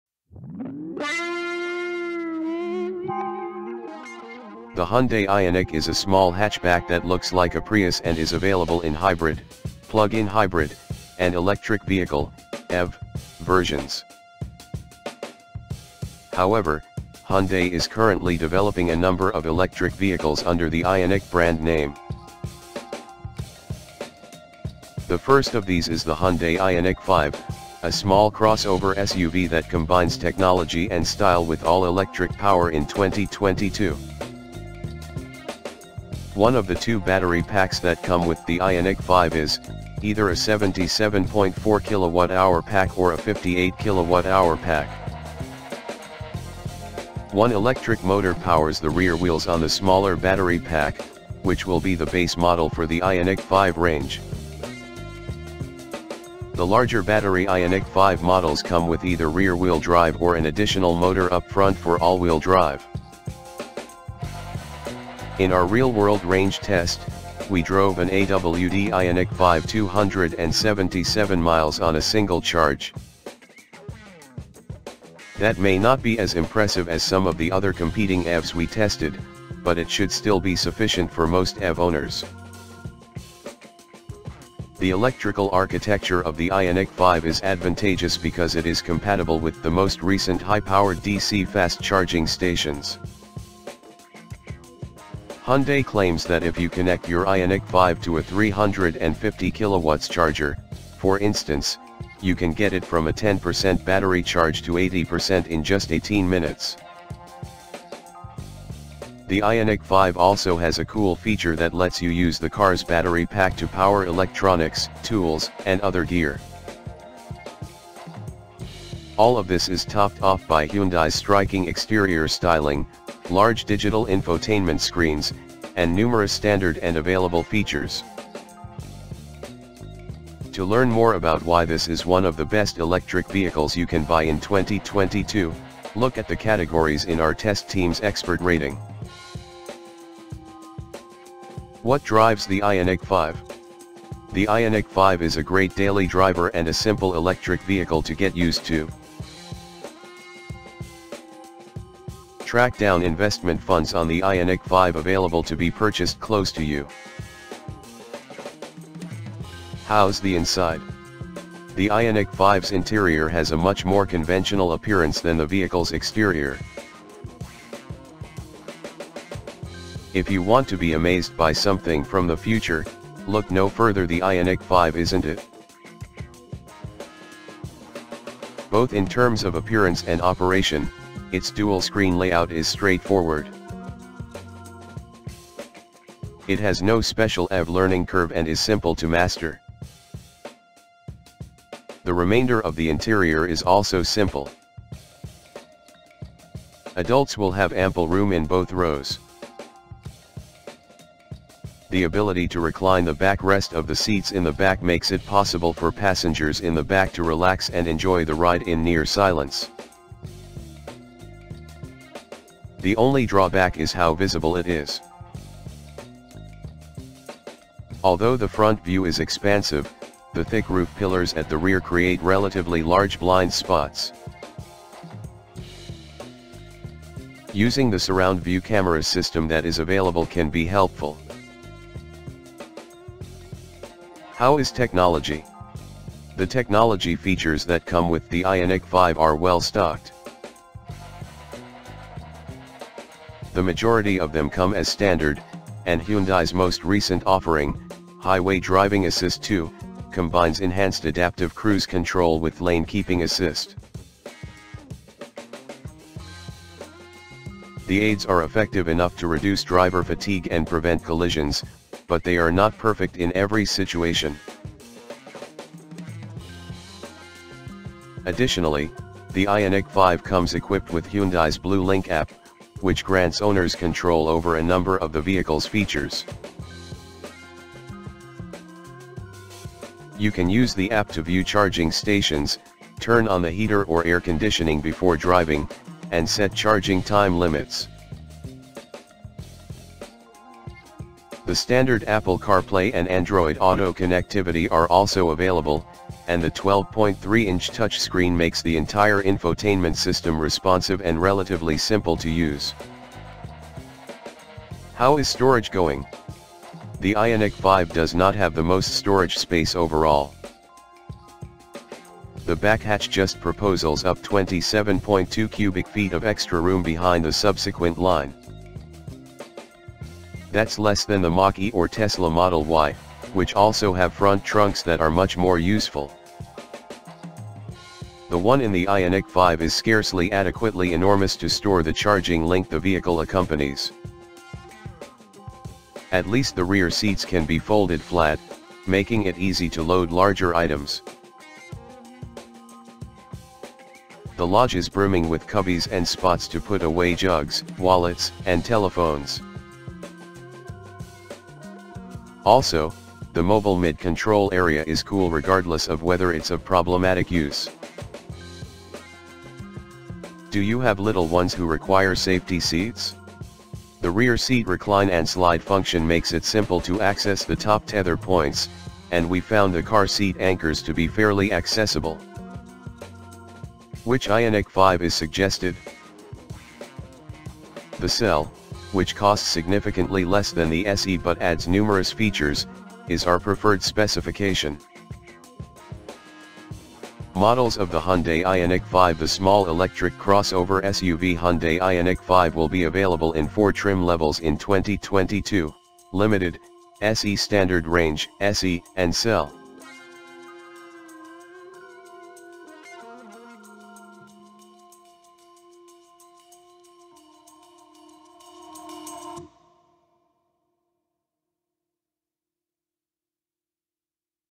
2022 Hyundai Ioniq 5 Limited AWD : An Electric Crossover SUV with Impressive Range and Versatility
The 2022 Hyundai Ioniq 5 is a small electric crossover SUV that offers impressive range, fast charging capabilities, and a versatile interior. It is available with two battery pack options and either rear-wheel drive or all-wheel drive. In our real-world range test, the Ioniq 5 drove 277 miles on a single charge. It also has a unique feature that allows it to power electronics and other gear using its battery pack. The Ioniq 5's interior is spacious and simple to use, with standard technology features such as Apple CarPlay and Android Auto connectivity and a range of advanced driving aids. Despite its small storage space, the Ioniq 5's rear seats fold flat to allow for larger items to be loaded. Overall, it is a strong contender in the electric vehicle market.